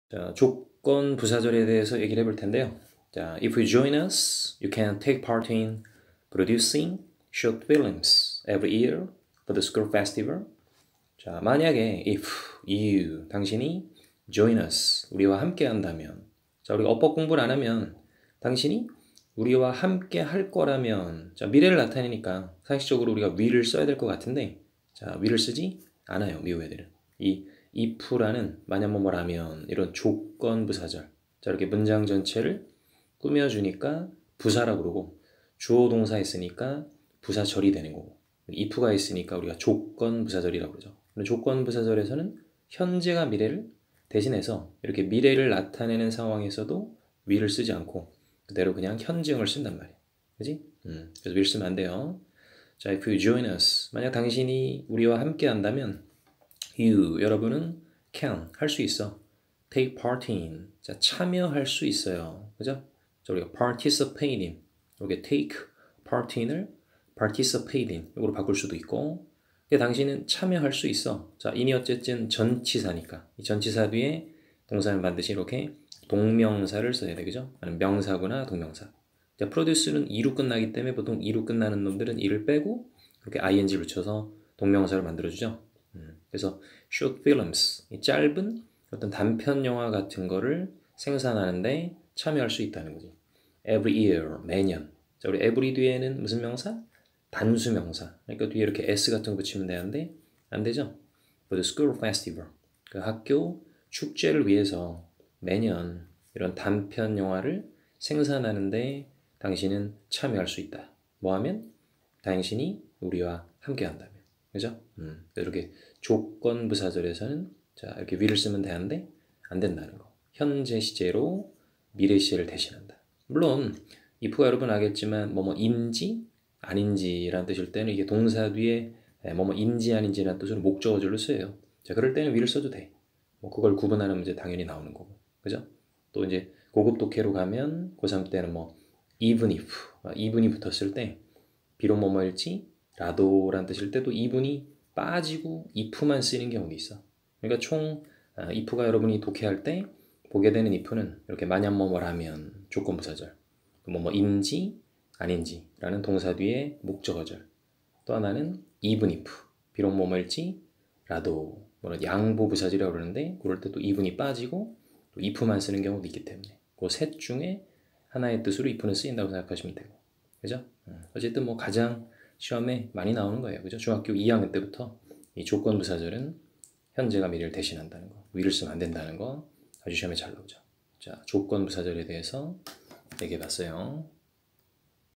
이종국 조건 부사절 영어 금성 최인철 1과 이그잼포유
조건 부사절에서는 현재시제가 미래시제를 대신해서 will을 쓰면 어법상 틀린다는 설명